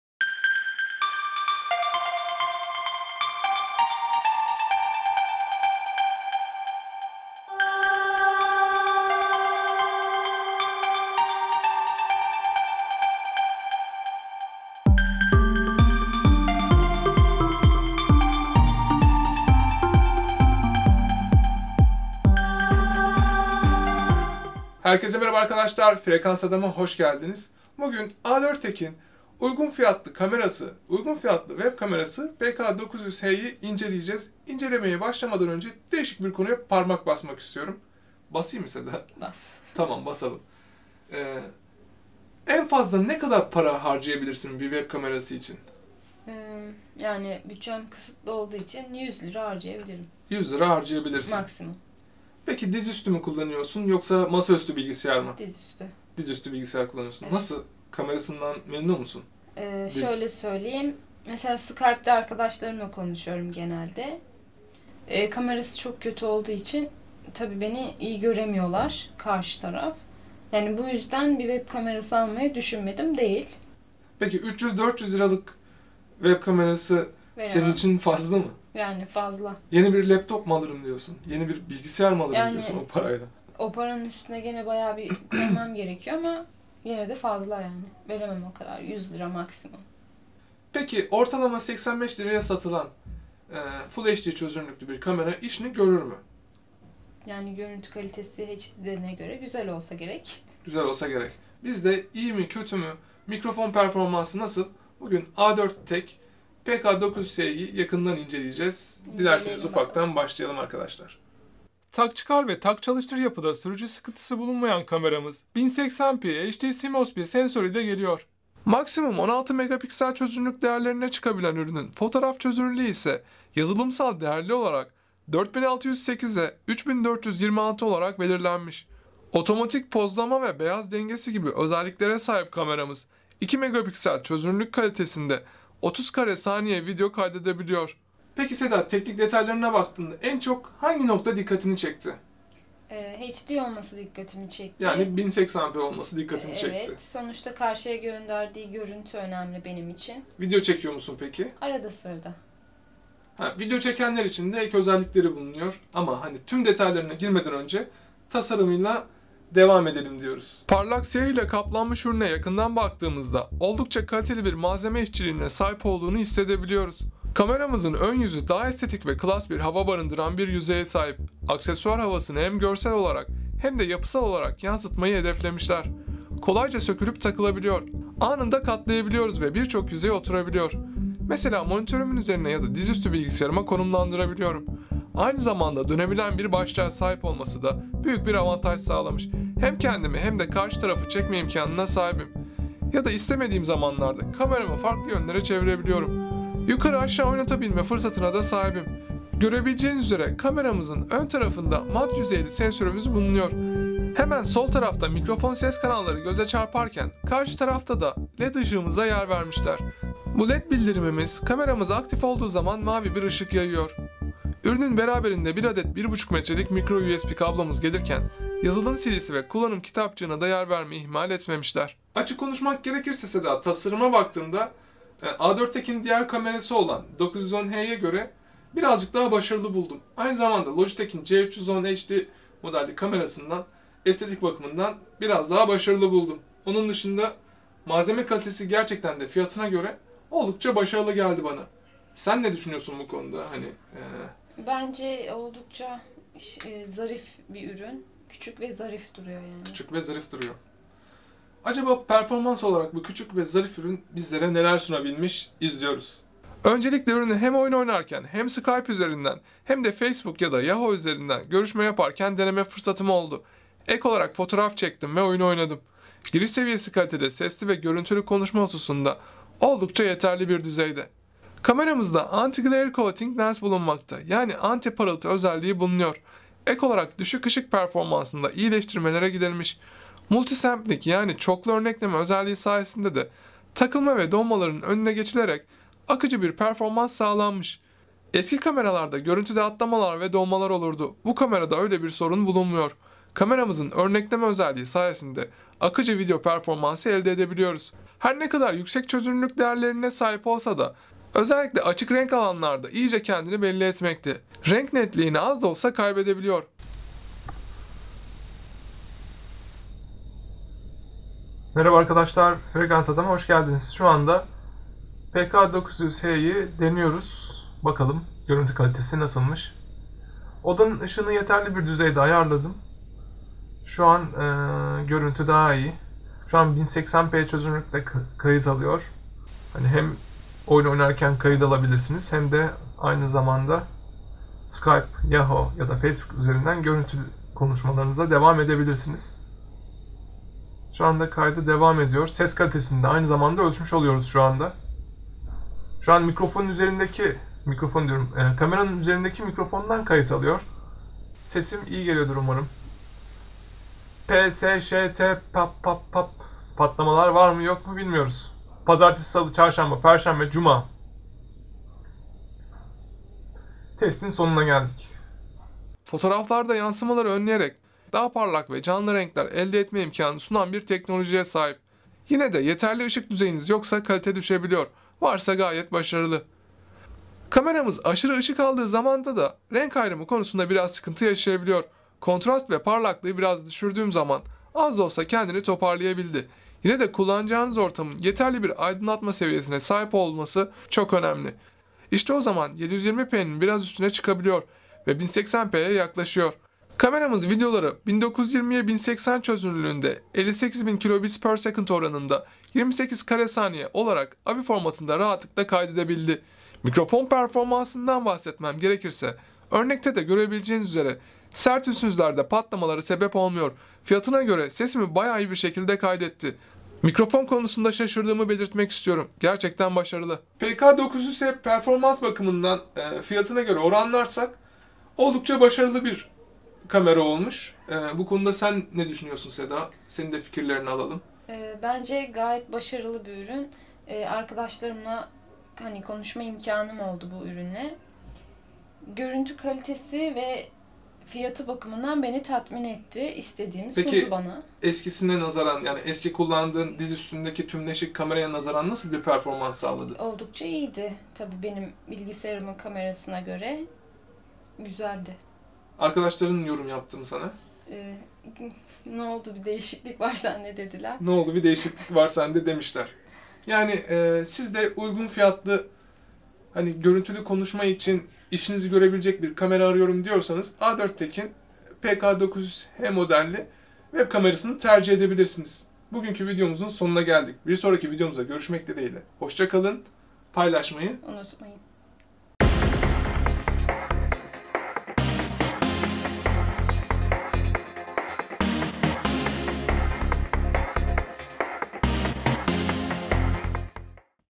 A4 Tech PK-900H 1080p Full HD-16MP Webcam İnceleme
Uygun fiyatlı webcam, hem yayın hem de oyun kamerası.
Sesli ve görüntülü konuşmanın yanı sıra oyunlarda kayıt imkânı sunabiliyor. A4Tech 1080p PK-900H, akranlarına nazaran başarılı, giriş seviyesi için ideal, hem dayanıklı hem de sesli ve görüntülü konuşma hususunda oldukça yeterli bir düzeyde.